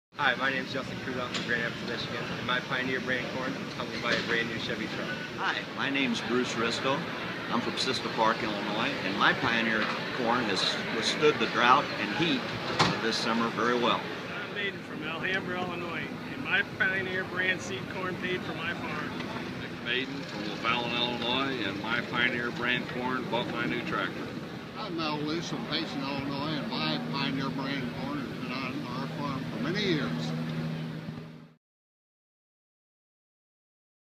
My Pioneer® brand corn: Withstood the Drought and Heat of the Summer
The seed a farmer chooses to sow is a personal choice. At this year's Farm Progress Show in Decatur, Illinois, we talked with growers about their experiences with Pioneer® brand corn. Here's what growers from Michigan and Illinois said.